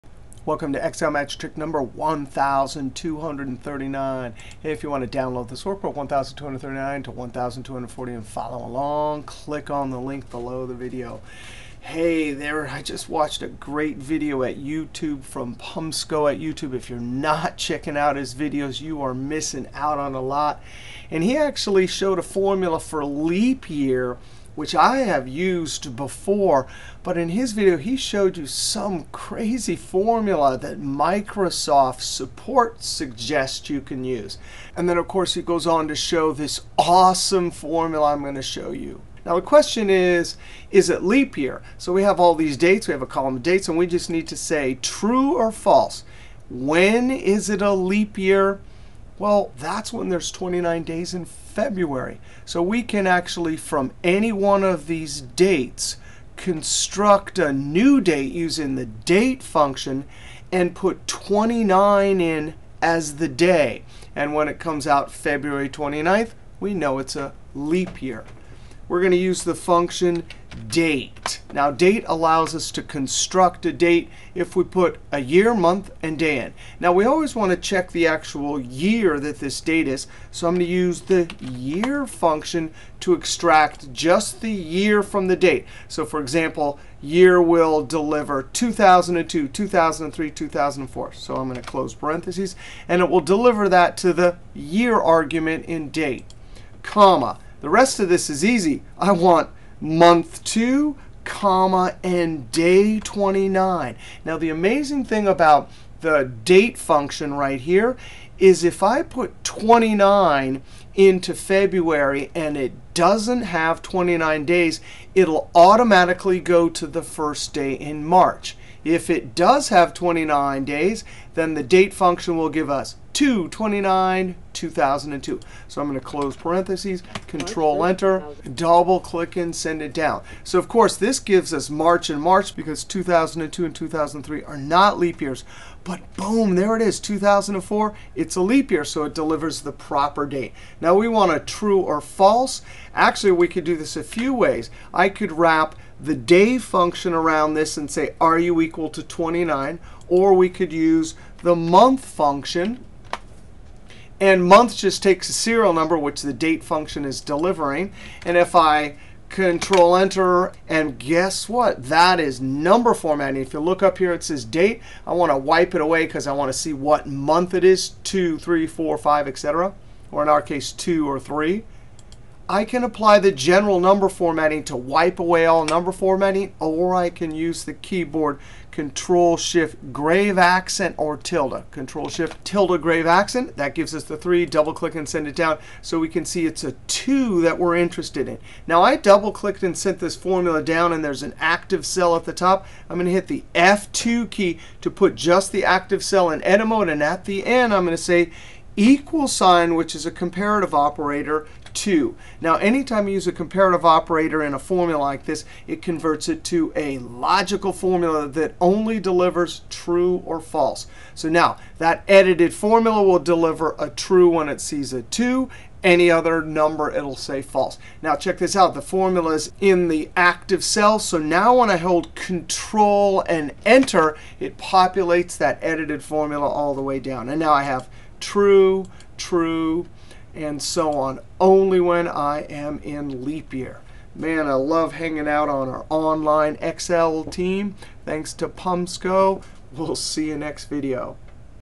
Excel Magic Trick 1239: Formula To Indicate a Date is in Leap Year
Learn how to create a Logical Formula that will deliver a TRUE or FALSE To Indicate a Date is in Leap Year. See the functions: MONTH, DATE and YEAR.